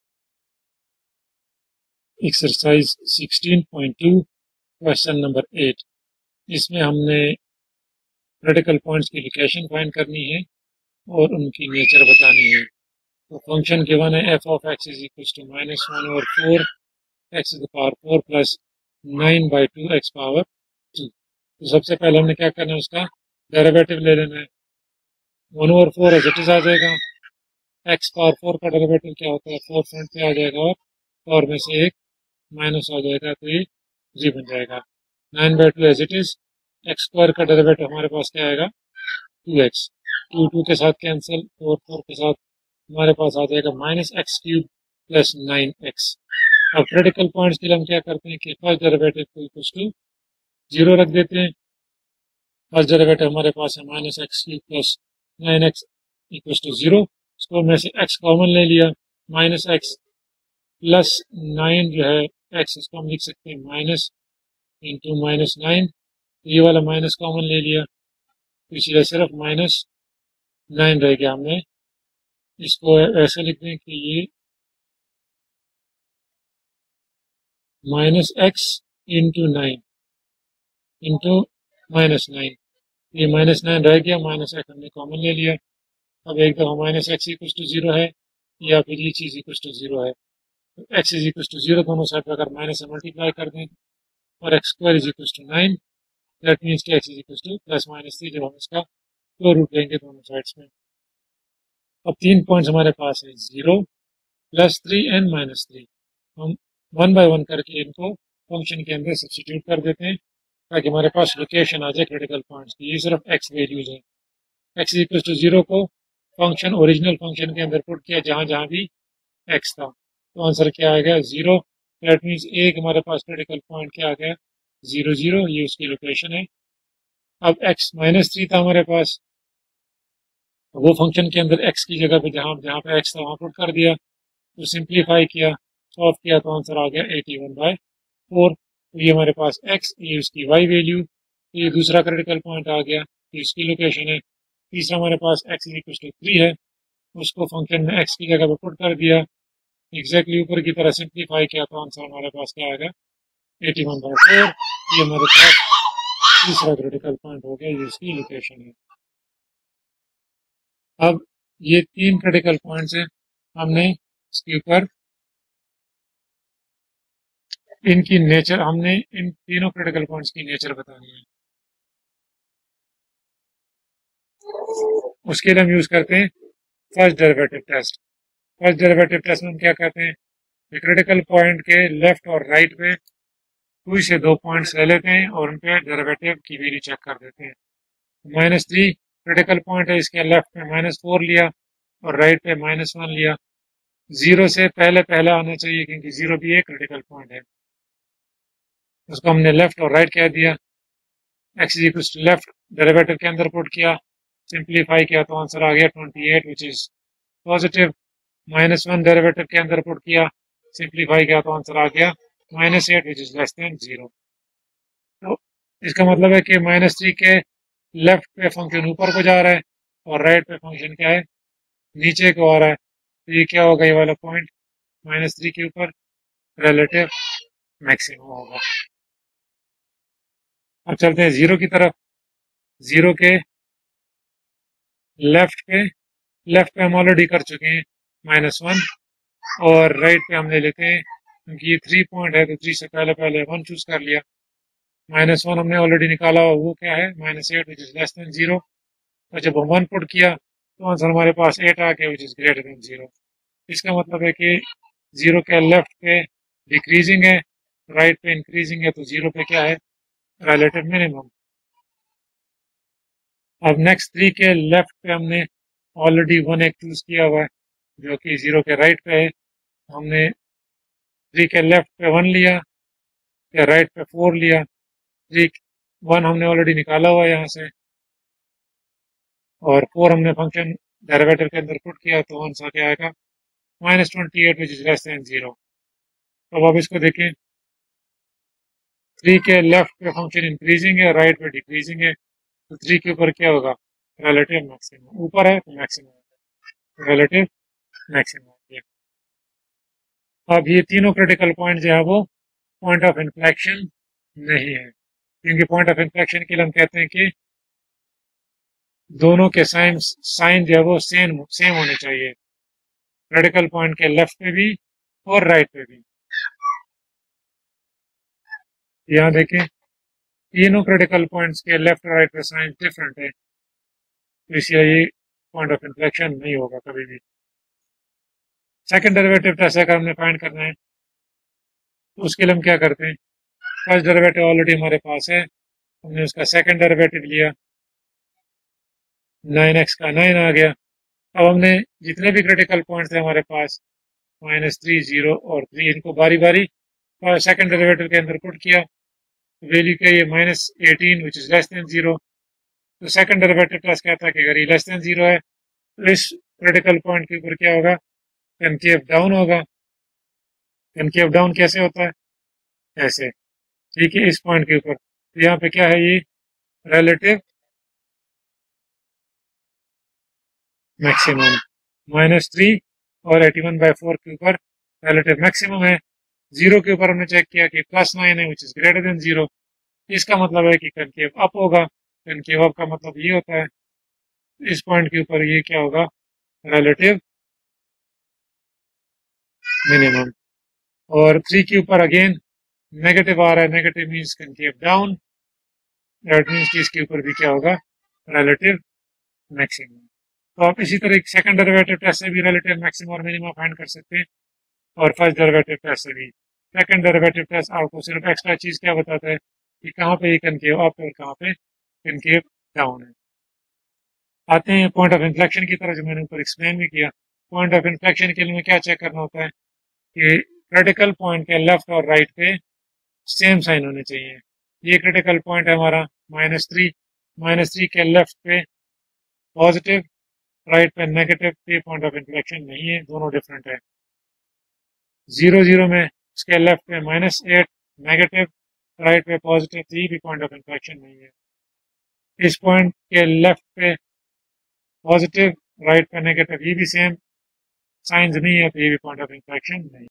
Business Math Ex # 16.2 Q # 8 Location & Nature of Critical Points Frank S Budnick 4th
This video explains the solution of Question 8 of Ex # 16.2 Location and Nature of Critical Points.

By: Frank S. Budnick
Edition: 4th
Section # 16.2